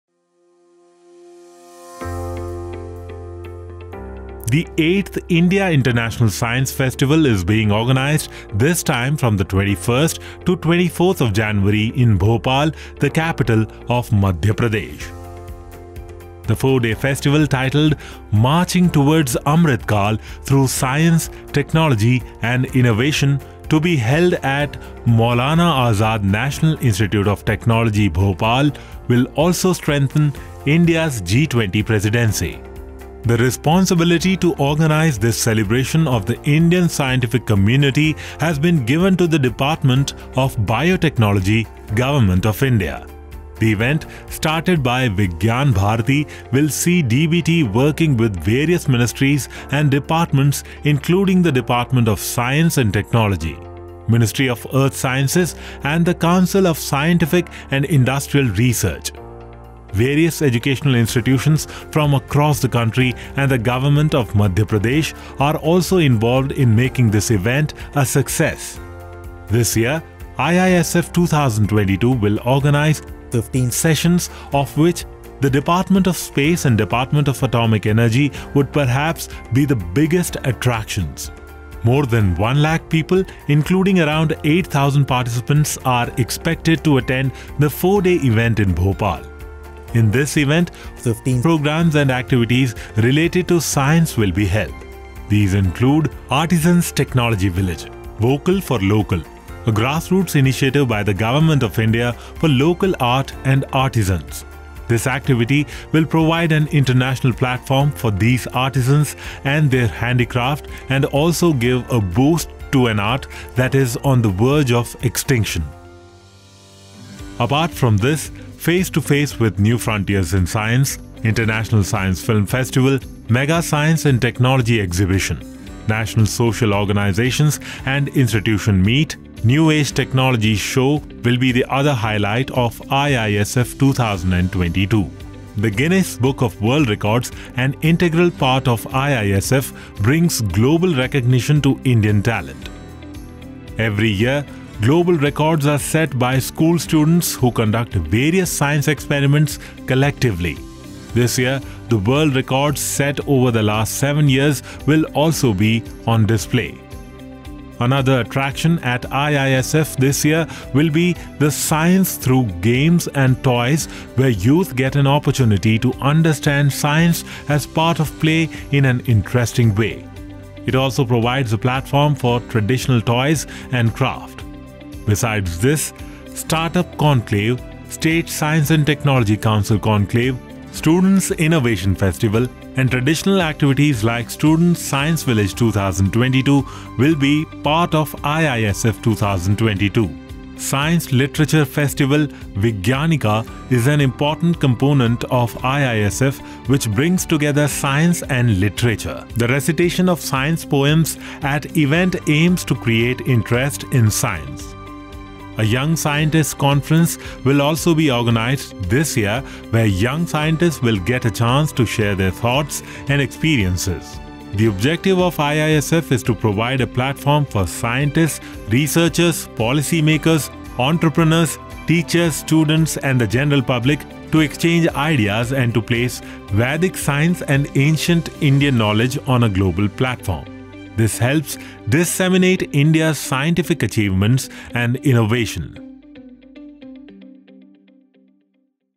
India International Science Festival – 2022
Eighth edition of IISF will be organized at Maulana Azad National Institute of Technology, Bhopal. The achievements of Indian science will be displayed by the science ministries and departments of the Government of India during this festival to be held from 21 to 24 January, 2023.